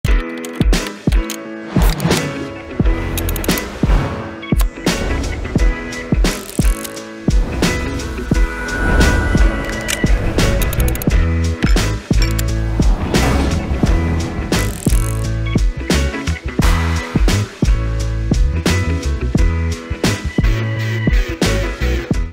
Ultimate iPad Tripod Mount
iPad devices are more powerful and more useful than ever. Mount them perfectly over your desk or within an open room to enable exciting new experiences. With VESA display arms, tripods, and Heckler Tripod Mount MX for iPad you can communicate and create in all-new ways.

👇 GET MORE HECKLER!